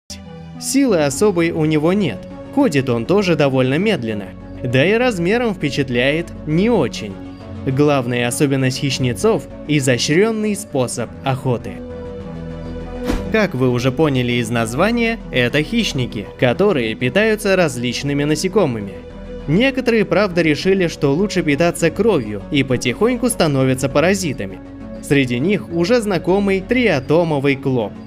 Клоп хищнец охотится /
- Заснял охоту двупятнистого клопа хищница (Platymeris biguttatus) на сверчка, под озвучку (трек) Знай ТВ.

I filmed a two-spotted assassin bug (Platymeris biguttatus) hunting a cricket.